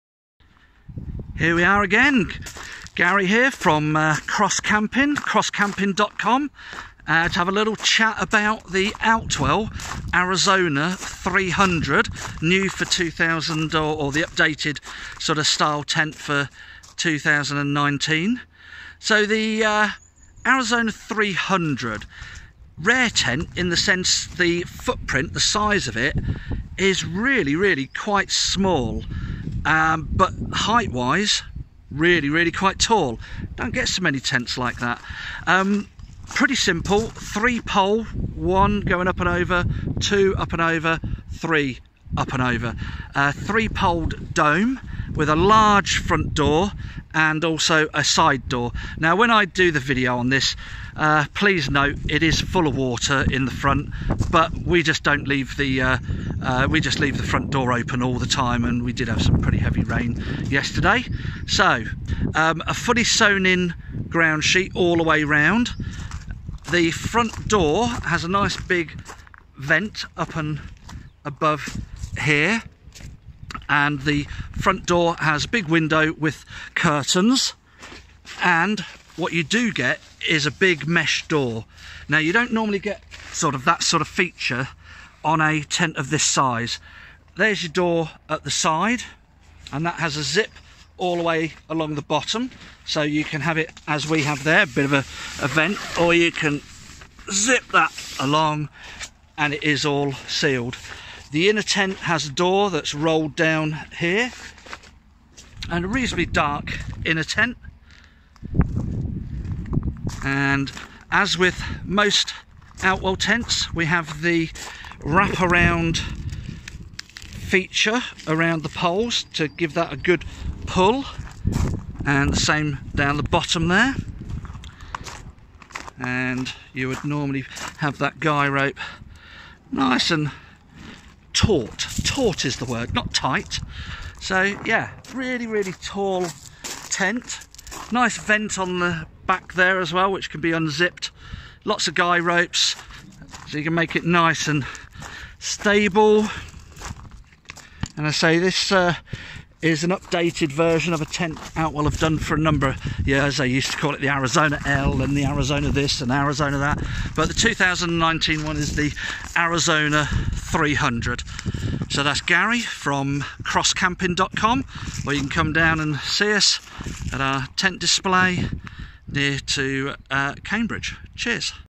Outwell Arizona 300 2019
The Outwell Arizona 300 brought to you is a rare style of tent. A small footprint area but tall enough for most campers to stand up in. Nice and comfy for 2 campers but officially a 3 berth.